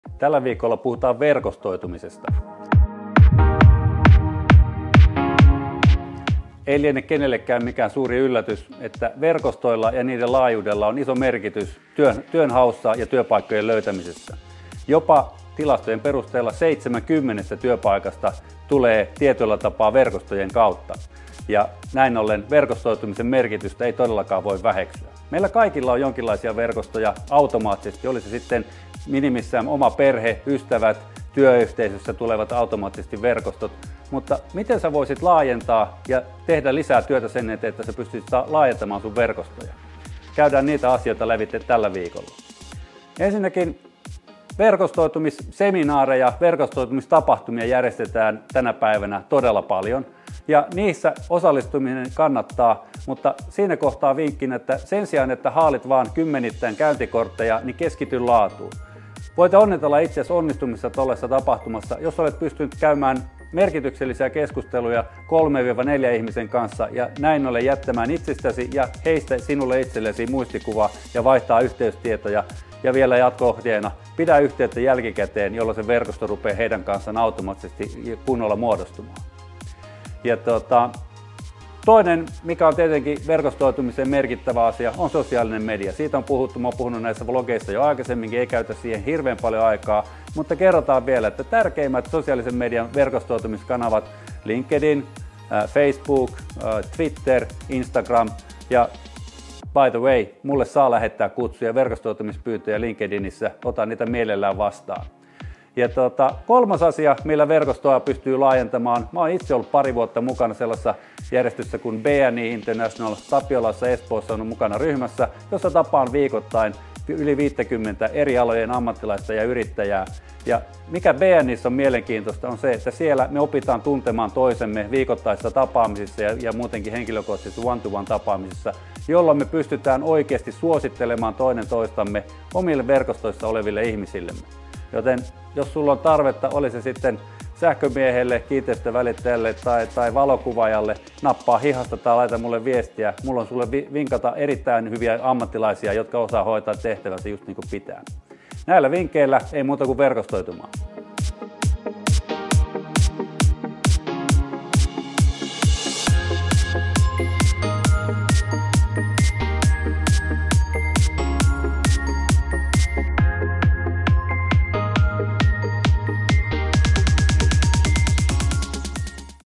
Kasvata verkostoasi näillä vinkeillä! | InHunt Group
Verkostojen merkitys työnhaussa on kiistaton, kun suurin osa työpaikoista menee erilaisten verkostojen kautta. Tällä videolla annetaan muutama vinkki siihen, miten pystyy kasvattaa omia verkostojaan.